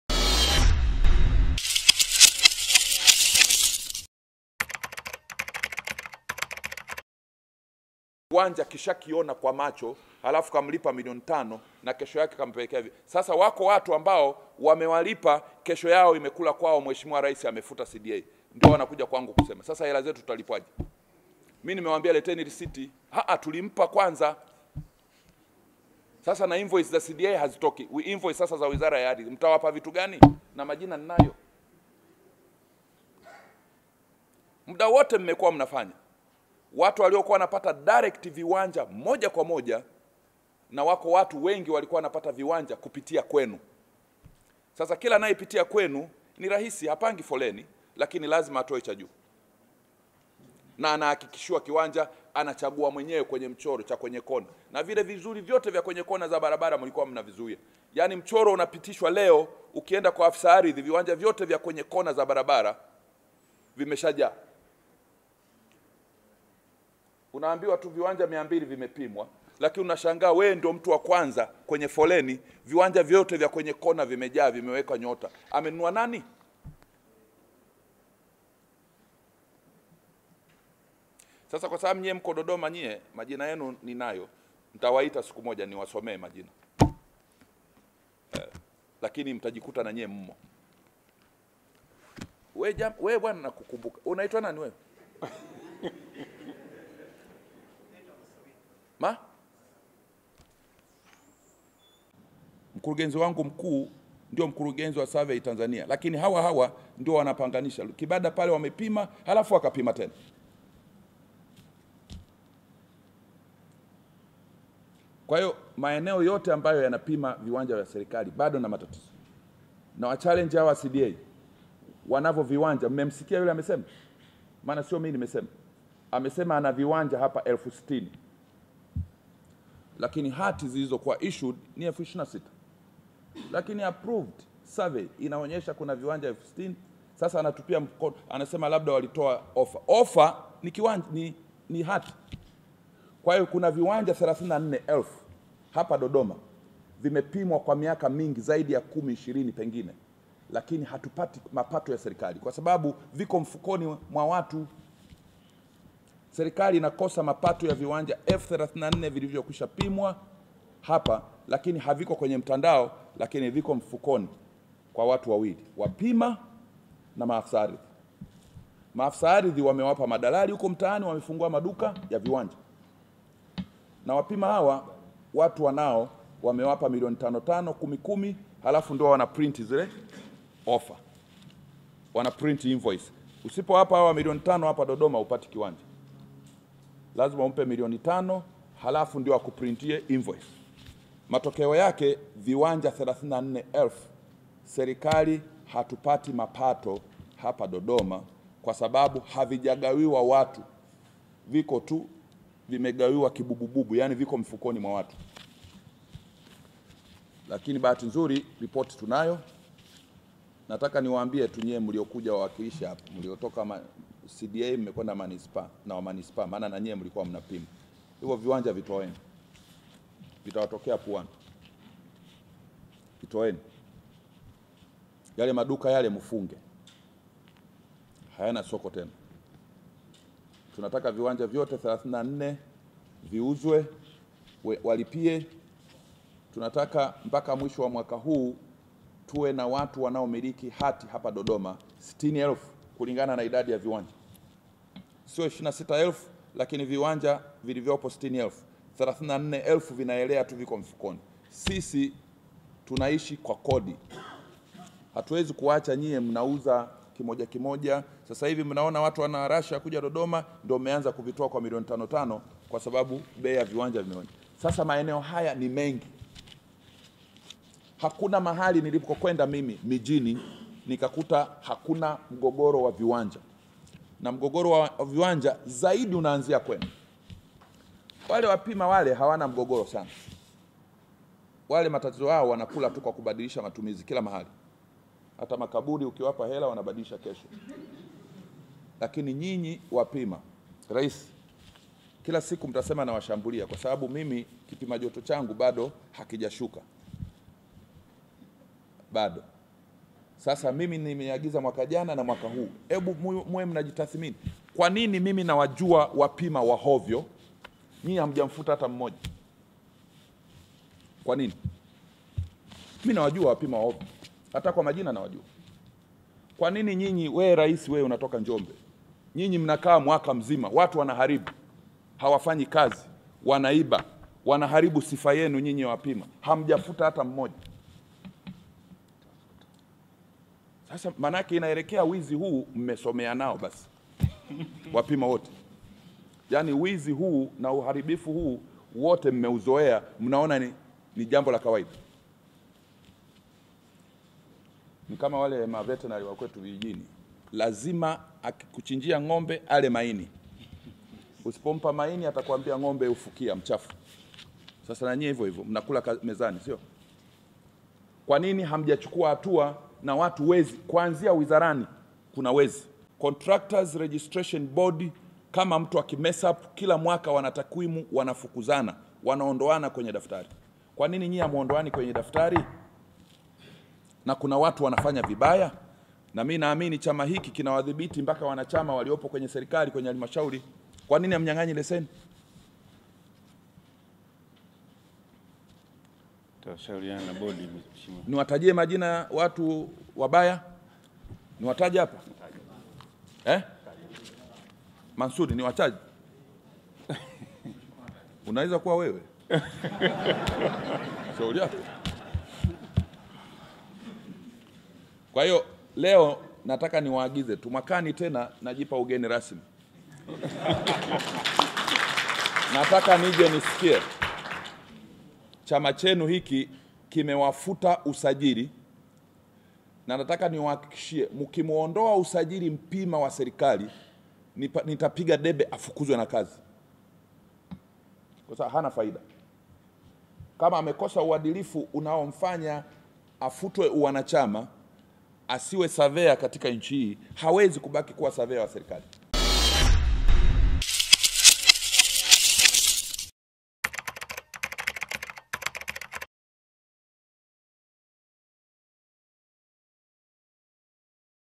Povu La Mhe William Lukuvi Kwa Wapima Viwanja
Waziri wa Aridhi Na Maendeleeo Ya Makazi  Mhe, Willim LuKuvi  Amekiagiza Chama  Cha Wakala Upimaji  Ardhi  Kuwafukuza  Mawakala  Wapimaji WaSiokuwa  Waaminifu Na Wanaosababisha  Migogoro Ya Aridhi

Aidha  Aliwataka  Waliokuwa Wafanyakaz Iliyokuwa  Malaka Ya Ustawishaji  Mji Mkuu Wa Dodoma CDA  Warudishe  Viwanja   Elf Sitini  Ambavyo  Avijulikani  Vilipo.